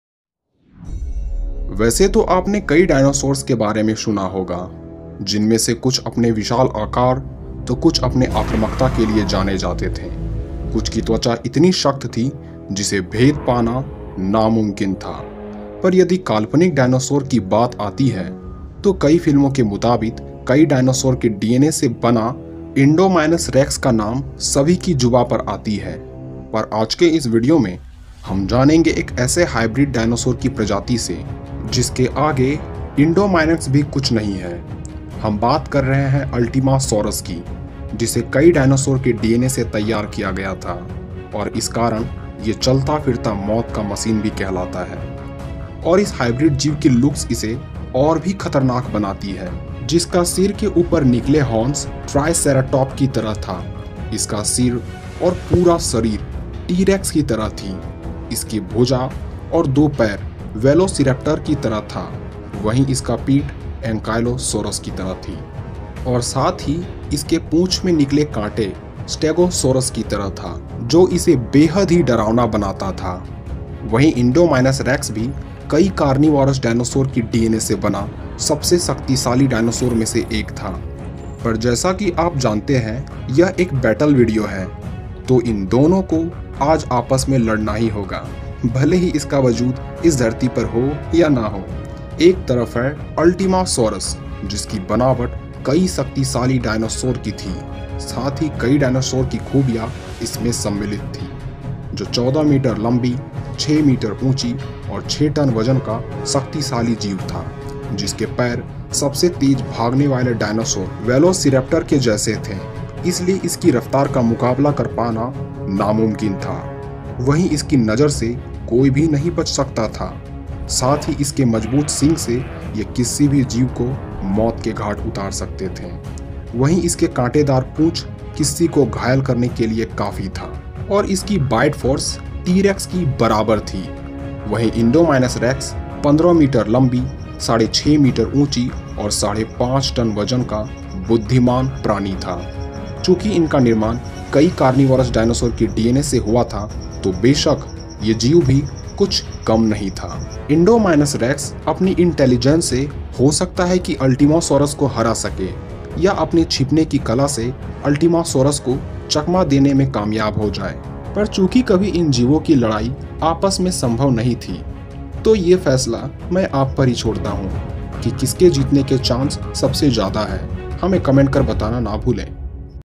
Indominus Rex Vs Ultimasaurus | Kaun Dinosaur hai sabse shaktishali | Battle video
As know the concept of Hybrid dinosaur from Jurassic world is amazing and people think of strongest dinosaurs we can made
yaa, it's seems funny and childish but this video is based on hypothesis for dino fans
so, in this video we'll see which dinosaur are more stronger
from one corner there is Indominus Rex also known as I Rex which is introduced in Jurassic world and made of many carnivorous Dinosaurs like Tyrannosaurus Rex (T-Rex), Spinosaurus, Velociraptor, etc.
while in another corner deadly Ultimasaurus roar which was made up of Many strongest dinosaurs
like Ankylosaurus, T Rex, Velociraptor, Triceratops, Stegosaurus and more which make it so deadly
so in this battle who would win?